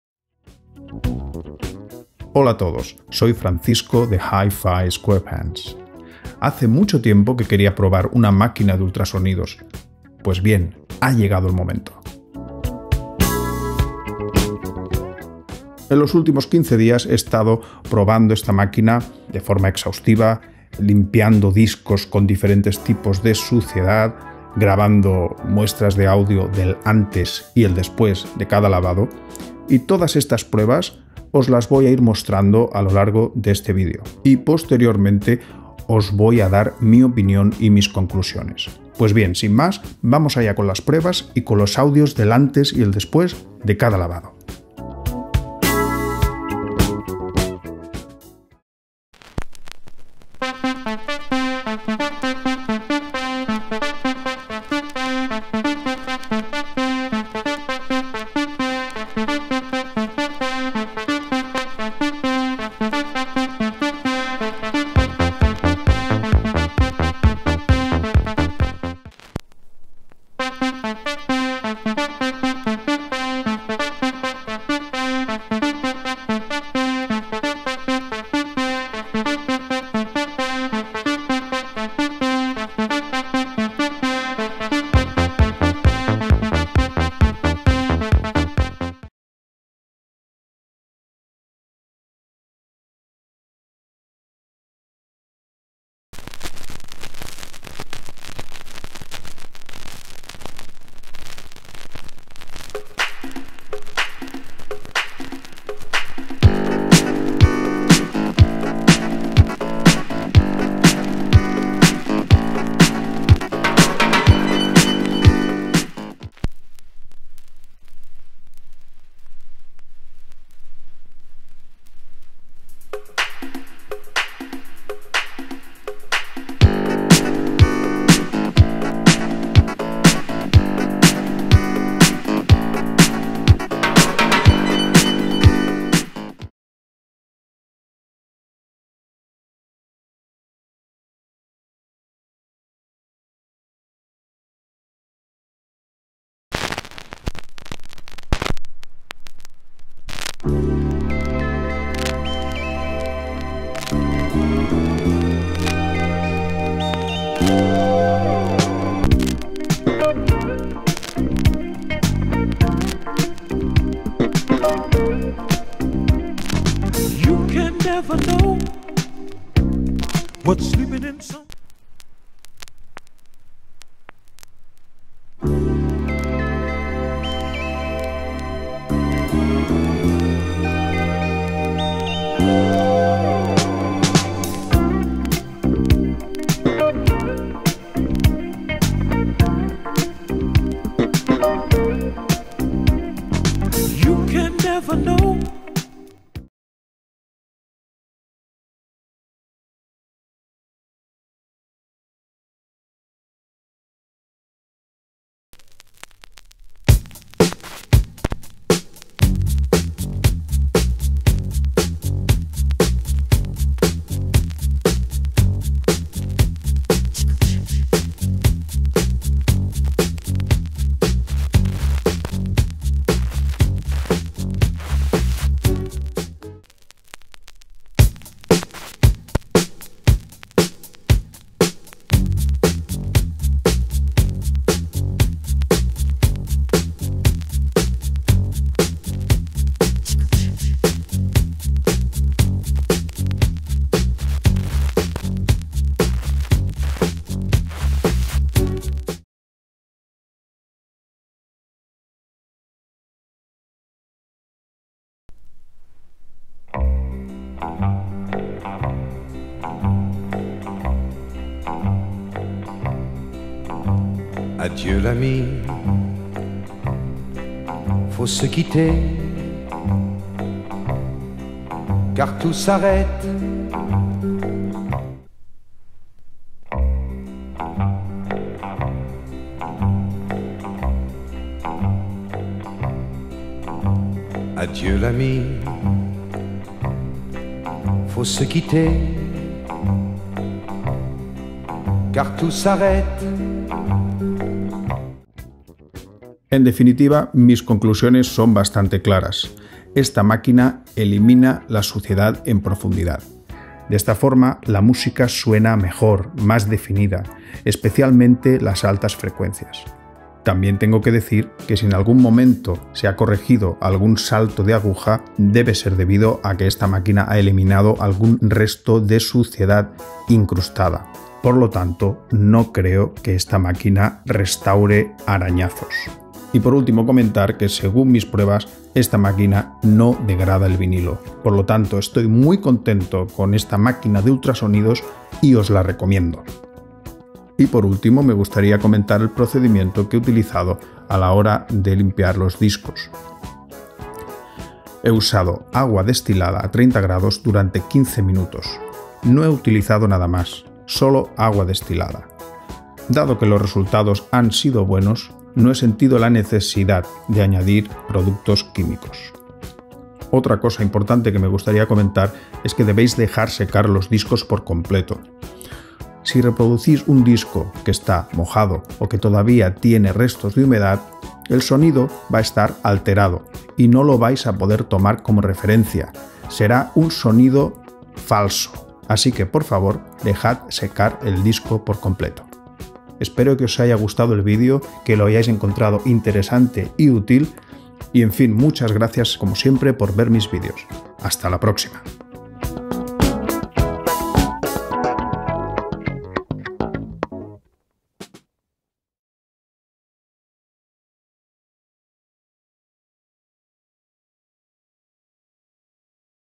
Ultrasonic Record Cleaning Machine - Review - Test - Opinion - Before / After Audio Clips
👉🏻Ultrasonic Record Washer:
US (incl. spinning mount)